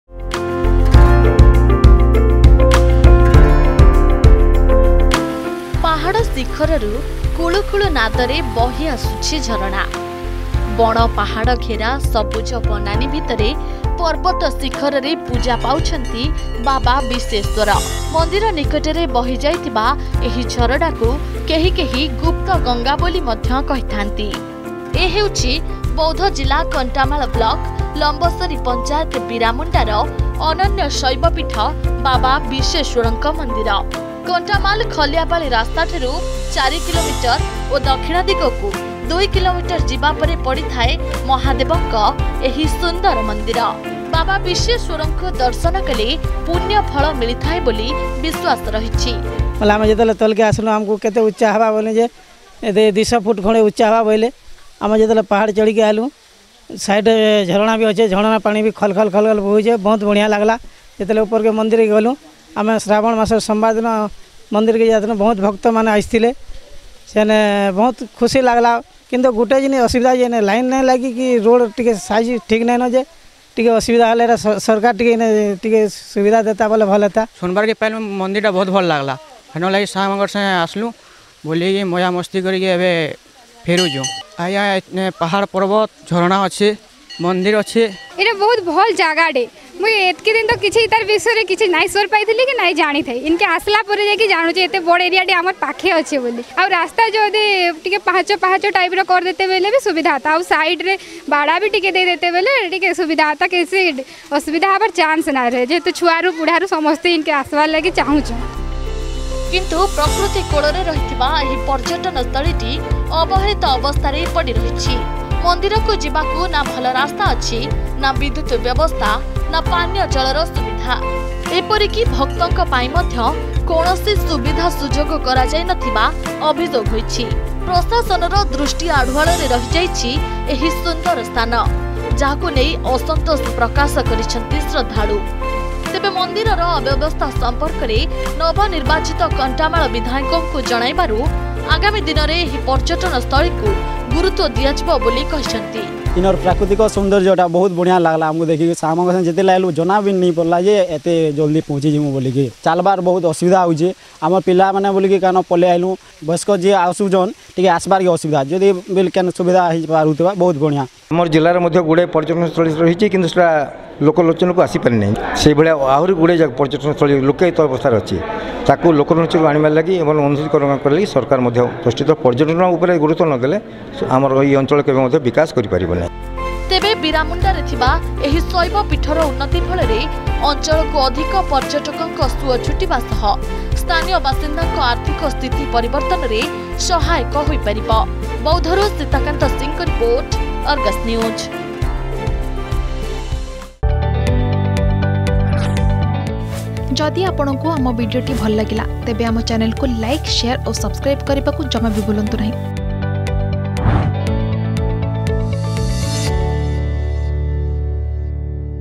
ପାହାଡ଼ ଶିଖରରୁ କୁଳୁକୁଳୁ ନାଦରେ ବହିଆସୁଛି ଝରଣା l|| Baba Bisheshwar Temple
ପାହାଡ଼ ଶିଖରରୁ କୁଳୁକୁଳୁ ନାଦରେ ବହିଆସୁଛି ଝରଣା l ବଣ ପାହାଡ଼ ଘେରା ସବୁଜ ବନାନି ଭିତରେ ପର୍ବତ ଶିଖରରେ ପୂଜା ପାଉଛନ୍ତି ବାବା ବିଶେଶ୍ୱର। ମନ୍ଦିର ନିକଟରେ ବହି ଯାଇଥିବା ଏହି ଝରଣାକୁ କେହି କେହି  ଗୁପ୍ତ ଗଙ୍ଗା ବୋଲି ମଧ୍ୟ କହିଥାନ୍ତି ।